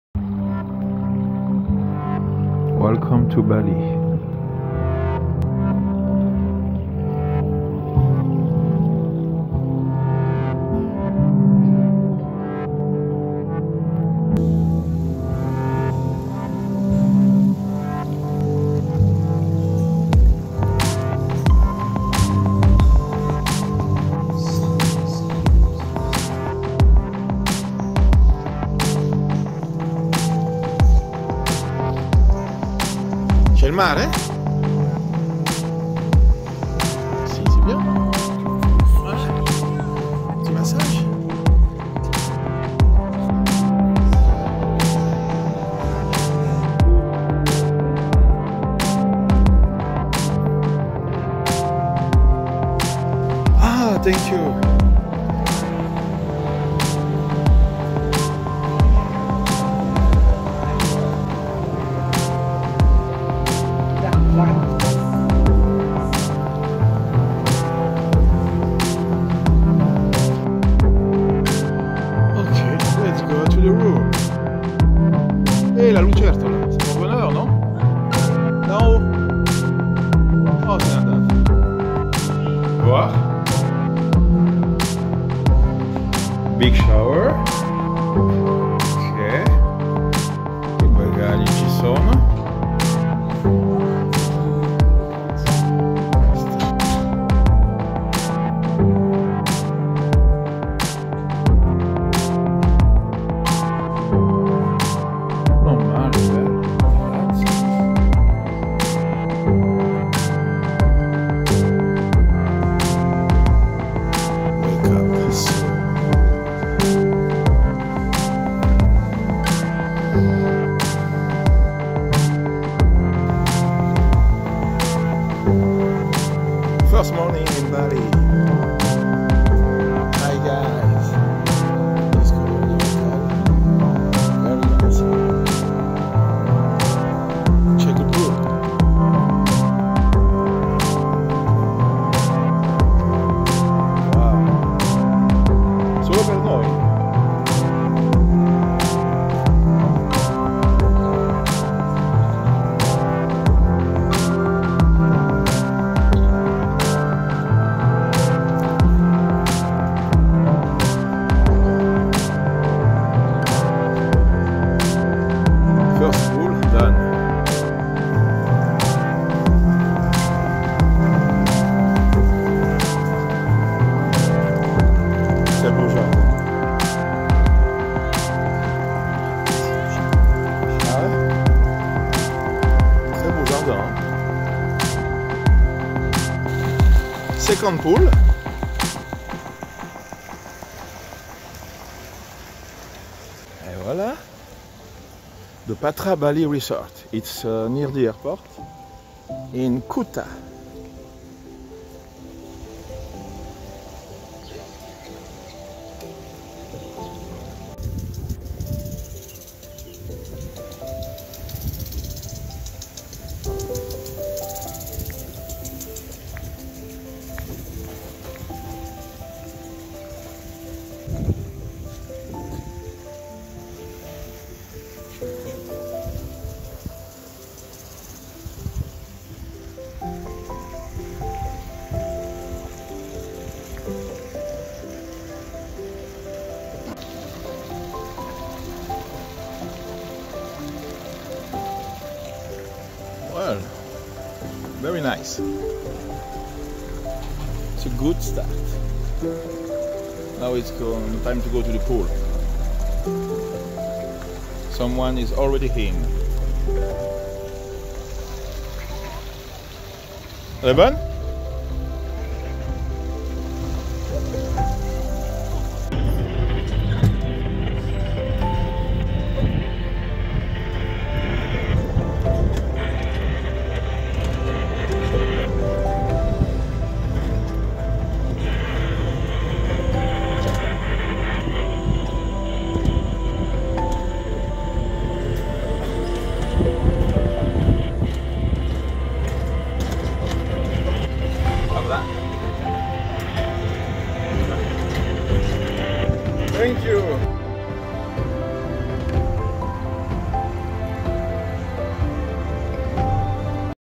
Bali Episode 1 : Kuta aera Patra Bali Resort and Spa
Visit this nice resort near airport
Very quiet, Very clean